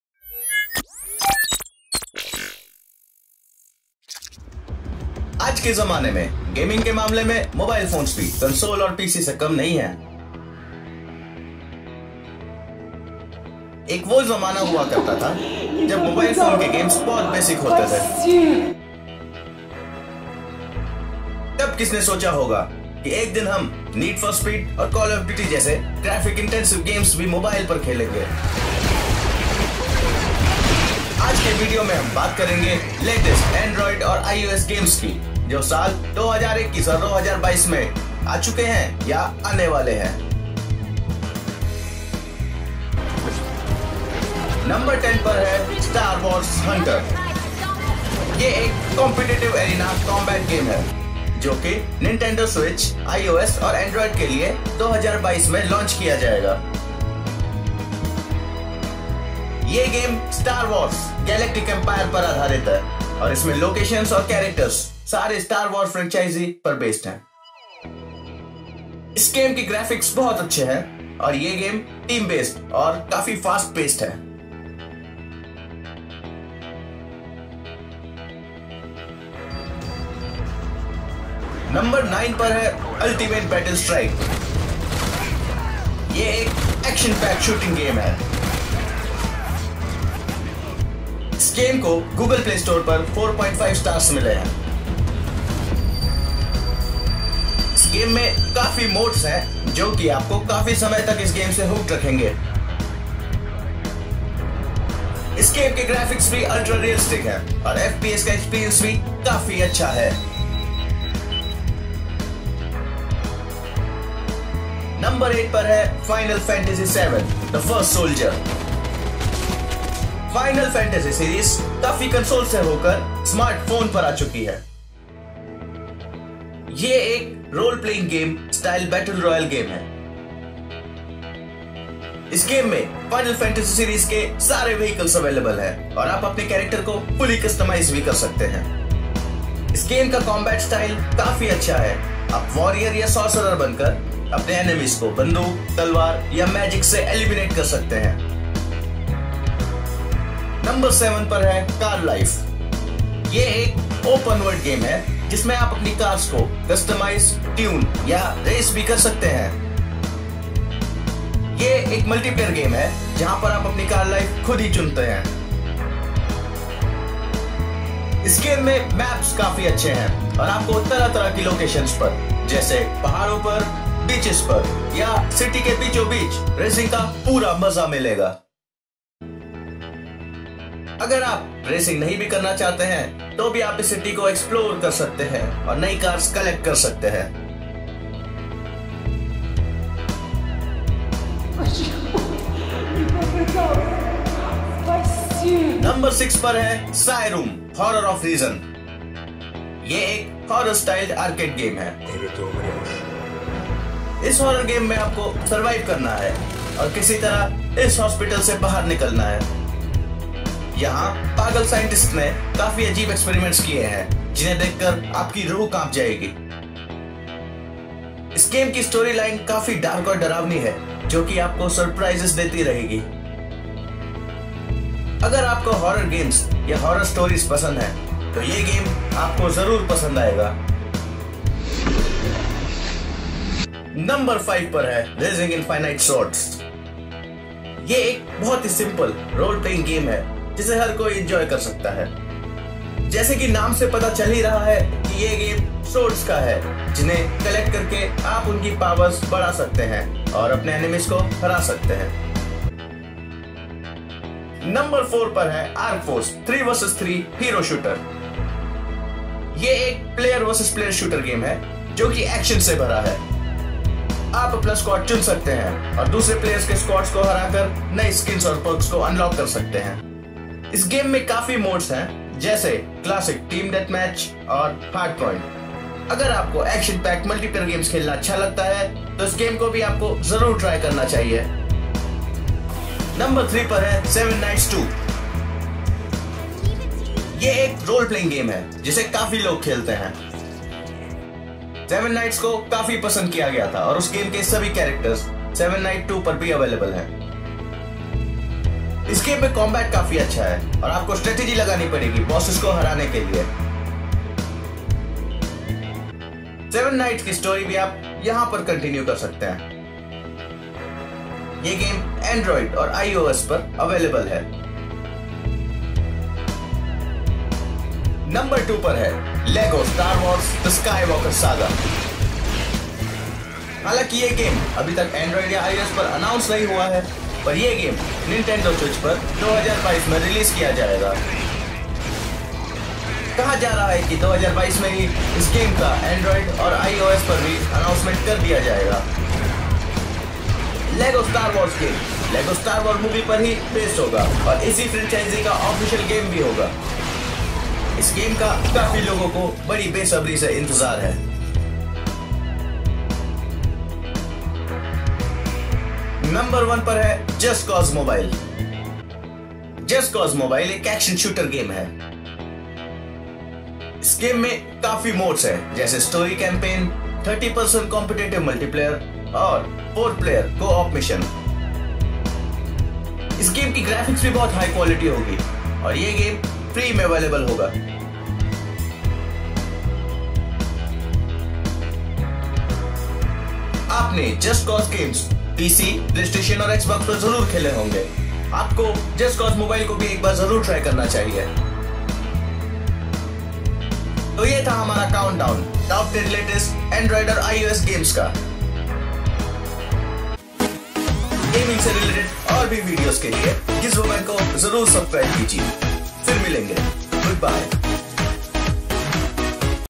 Top 10 Latest Android and iOS Games Reviews (2021-2022)
There are a lot of new games coming out in 2021-22 for Android and IOS. Here are all of the games, reviews that you should be aware of.